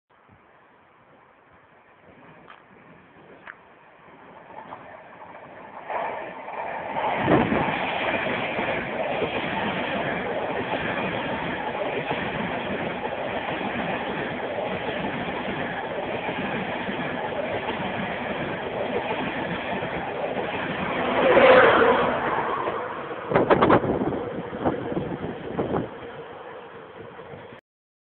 91125 passes through Manors
East Coast class 91 91125 passes through Manors in Newcastle